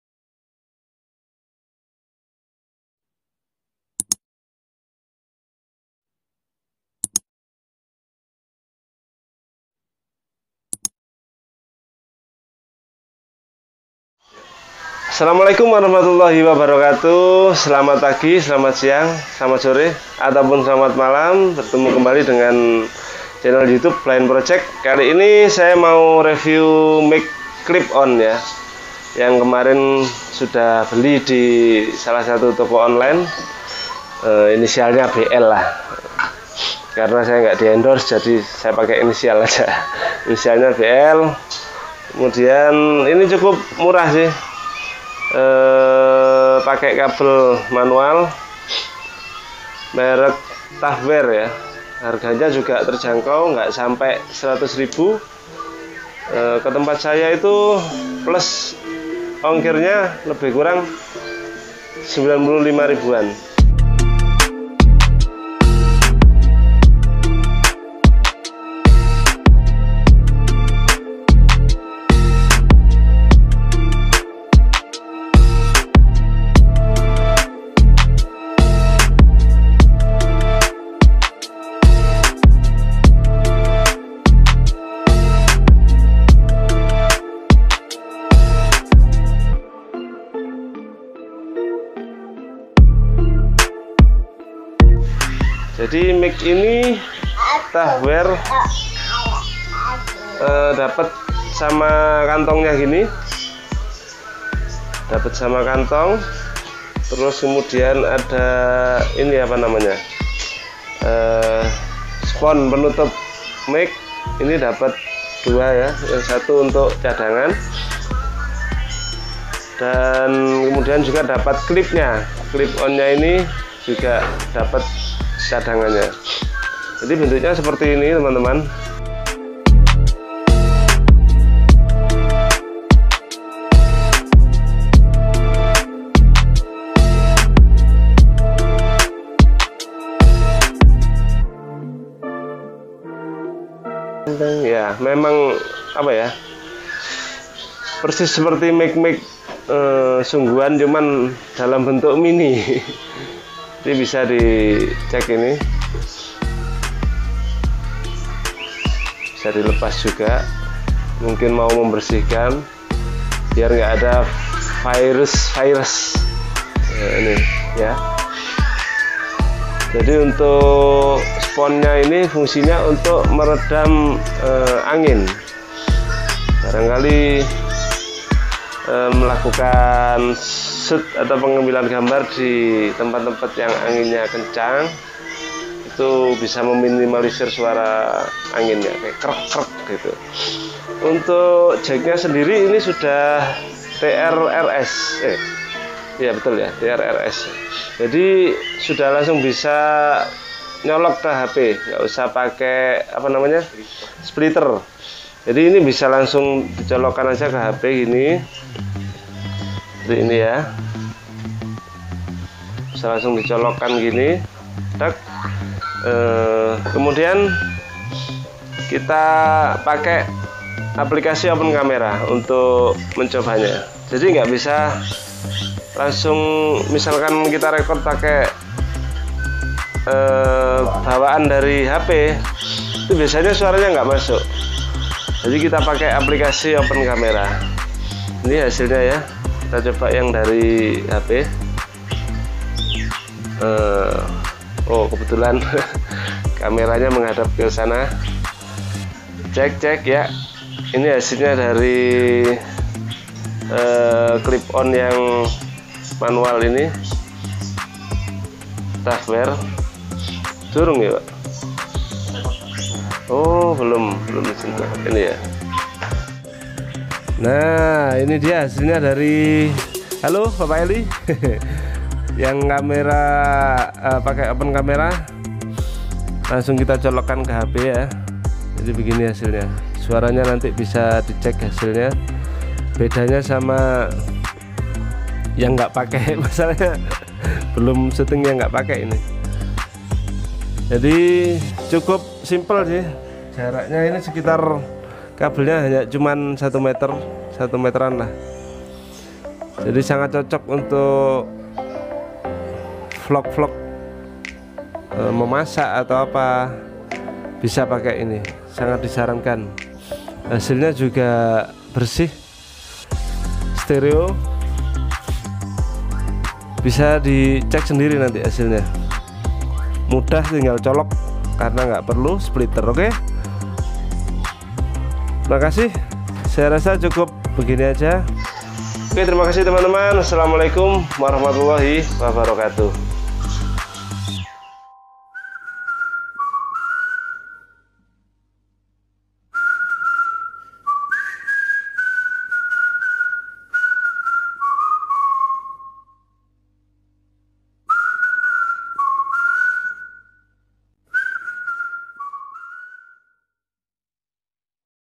Mic Clip On Murah Tapi Bukan Murahan || Taffware
Harganya gak bikin kantong tipis, terbilang cukup murah tapi bukan murahan. Mic clip on taffware